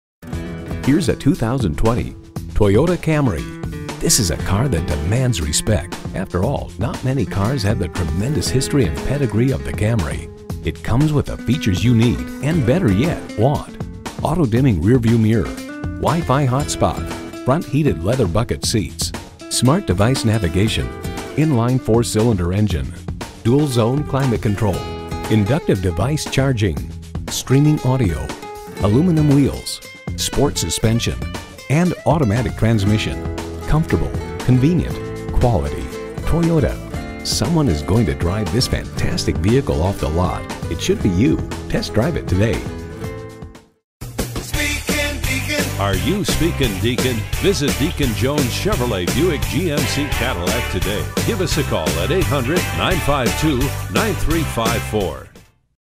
2020 Toyota Camry Smithfield NC Selma, NC CG32102A - SOLD
Year: 2020
Make: Toyota
Model: Camry
Trim: XSE
Engine: 4 2.5 L
Transmission: Automatic
Color: Pearl
Interior: Ash
Mileage: 68303
Stock #: CG32102A
VIN: 4T1K61AK5LU356367

Address:
1115 North Bright Leaf Blvd